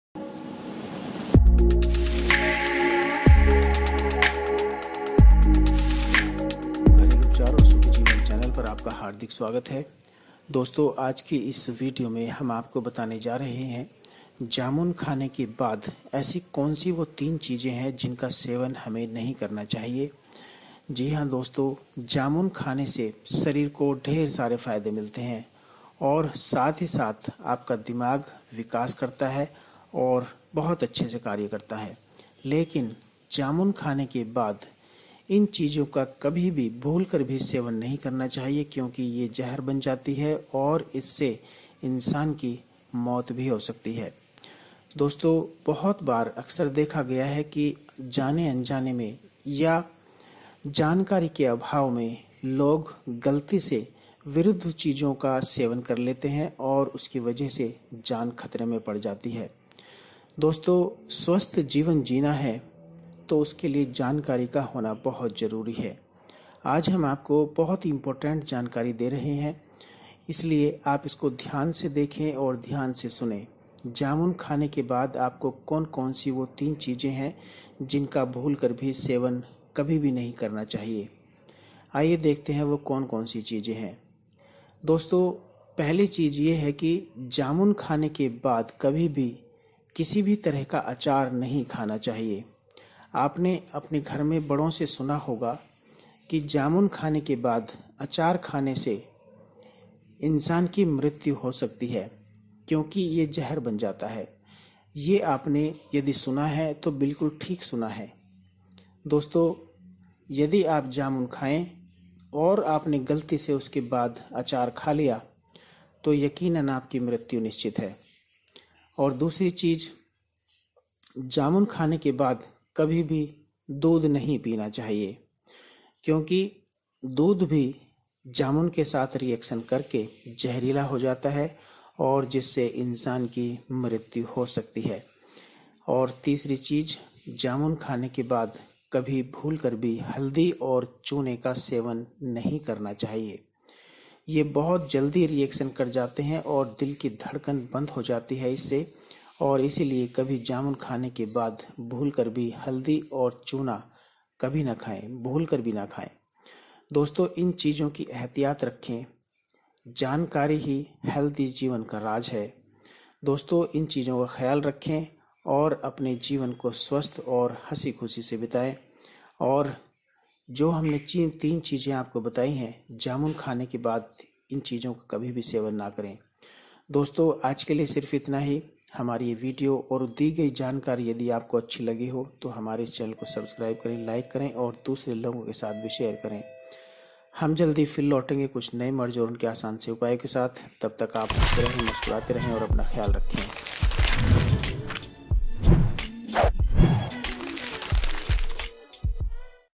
जामुन खाने के बाद भूलकर भी ना खाएं ये 3 चीजें वरना बन जायेगा जहर /Never Eat 3 Foods With Blackberry
जामुन खाने के बाद भूलकर भी ना खाएं ये 3 चीजें वरना बन जायेगा जहर / Never Eat These 3 Foods With Blackberry / Do No Eat These 3 Thing After Jamun / Worst Food Combination